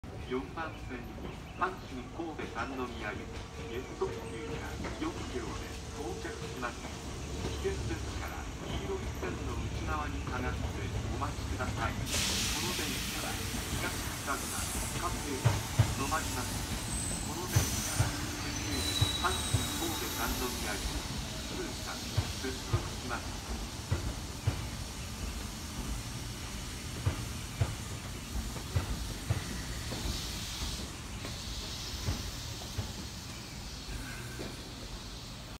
山陽3050系3次車(3060F)S特急阪神神戸三宮行き 高砂駅入線
7時36分入線

抵抗制御＋三菱電気